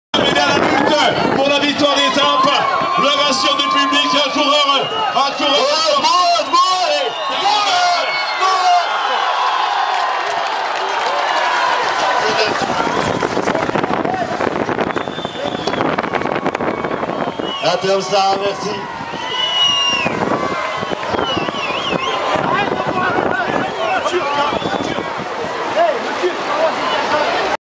Arrivé 2éme 2tape - Sétif Biskra Le 12 Mars 2013
Radio Algérienne _ Cyclisme -- TAC 2013 _ Sergiy Grechyn roi des Zibans (audio-vidéo)